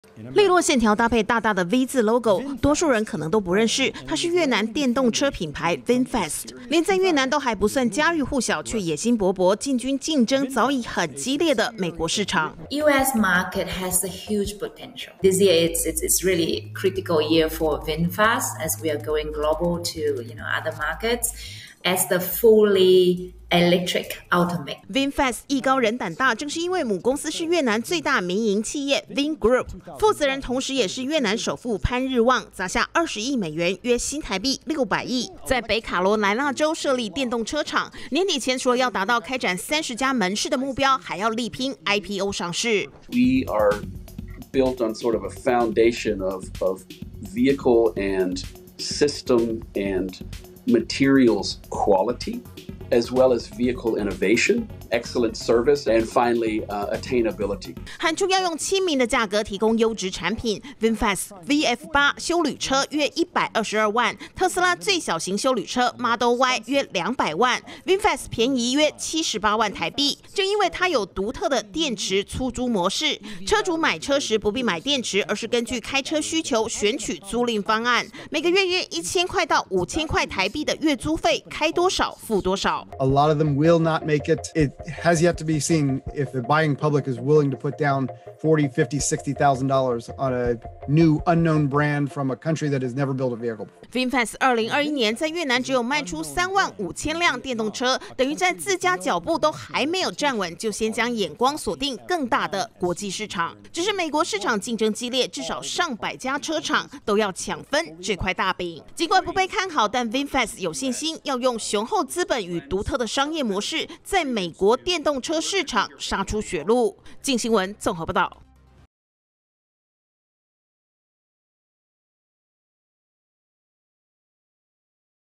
越南新電動車進軍美國！ 9月產線投入拚年底交付｜鏡新聞
電動車特斯拉有強敵了嗎？看準電動車未來商機，越南最大民營集團Vingroup，它旗下的電動車公司VinFast，砸下600億台幣，在美國當地設廠，工廠9月投入產線後，要力拚年底前，完成首批電動休旅車的交付。面對競爭激烈的美國電動車市場，VinFast依舊展現十足野心，要用比特斯拉更親民的價格，走出自己的路。